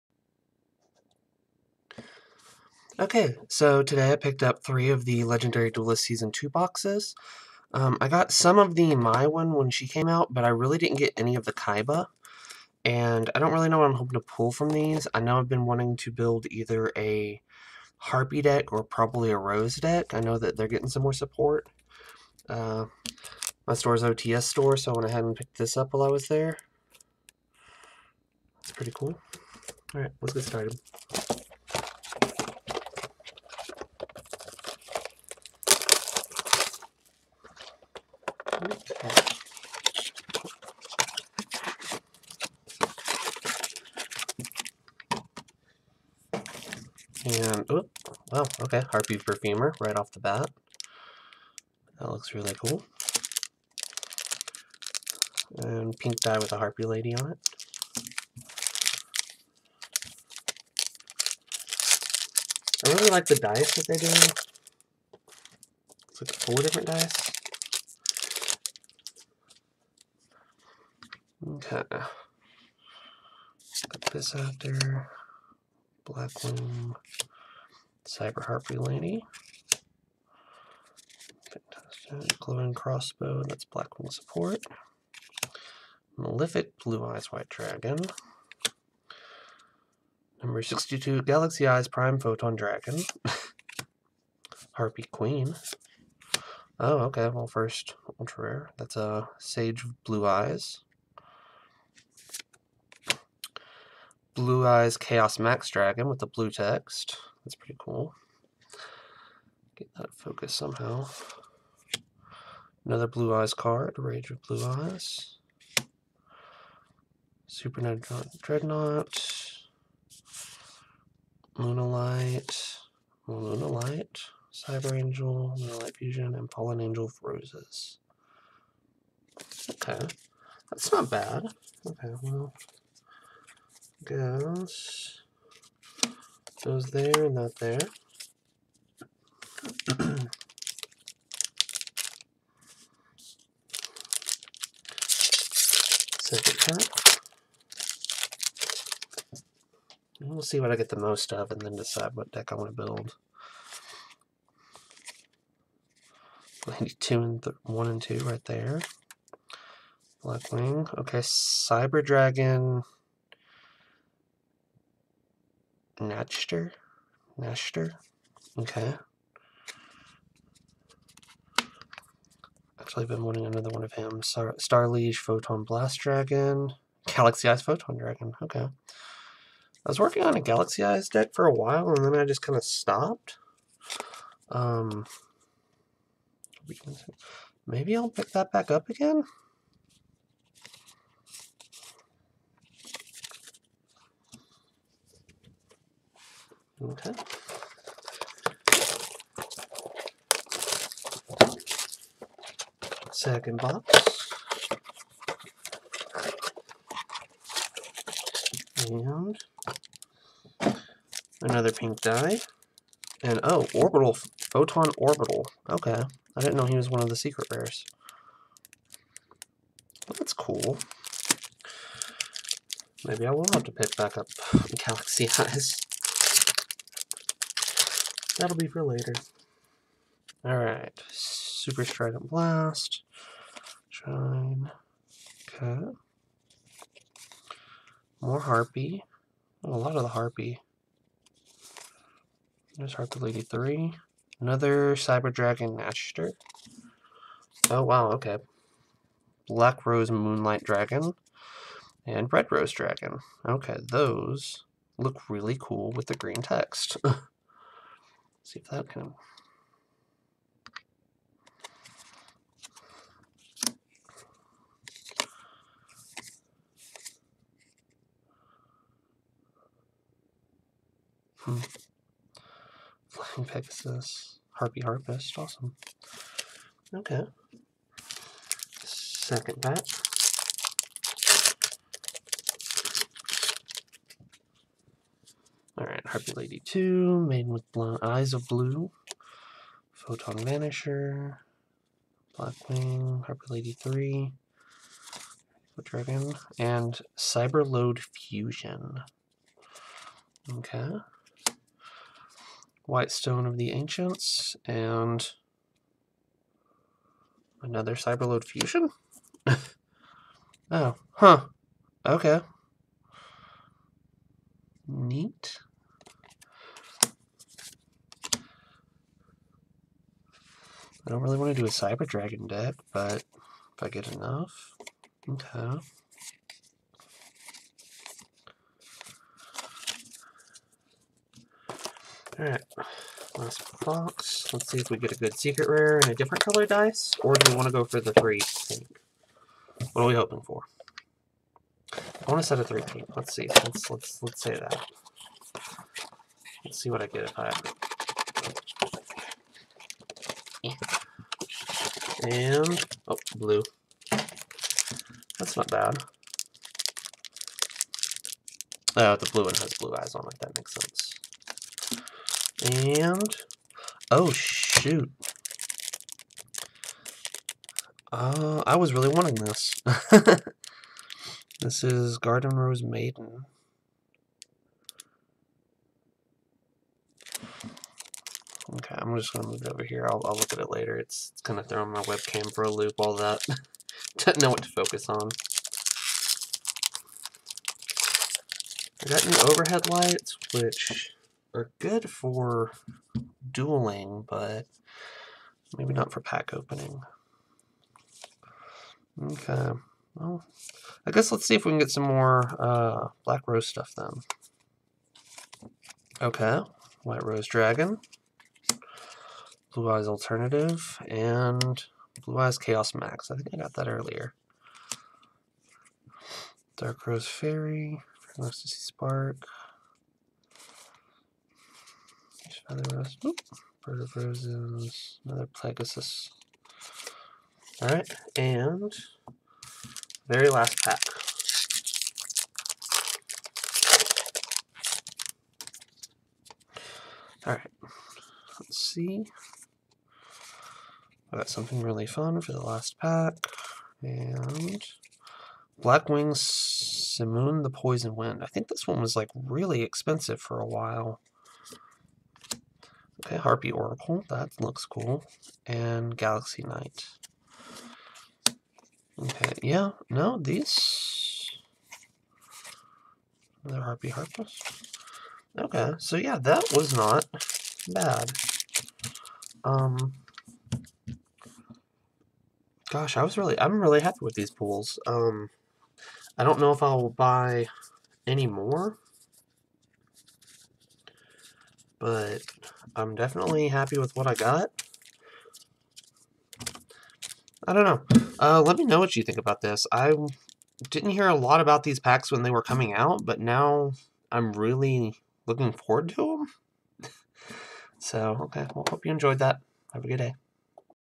Legendary Duelists- Season 2 box opening
I bought 3 boxes of Legendary Duelists season 2 and then I opened them...